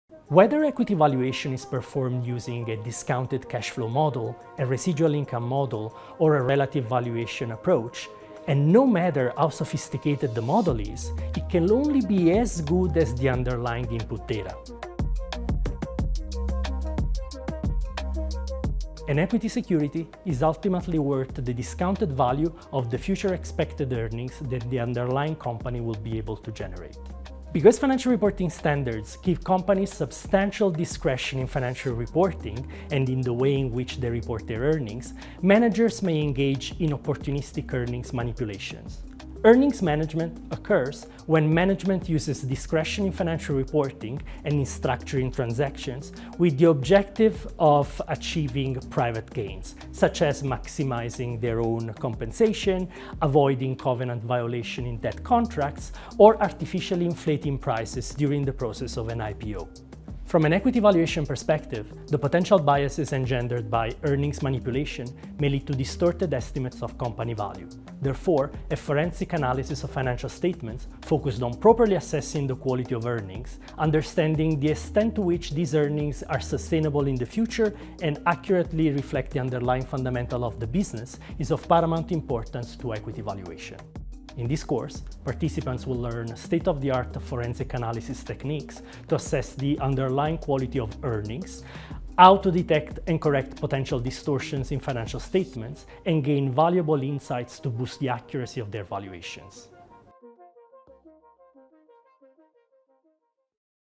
How to detect Earnings Manipulation | Financial Statement Analysis and Equity Valuation
Dr Stefano Cascino gives a brief overview of how the quality of financial information reported by corporations affects their valuation and how forensic analysis techniques can be used to detect earnings manipulation, so you can accurately interpret the data available.

Detecting earnings manipulation is a key topic covered in LSE’s new Executive Education programme ‘Financial Statement Analysis and Equity Valuation’.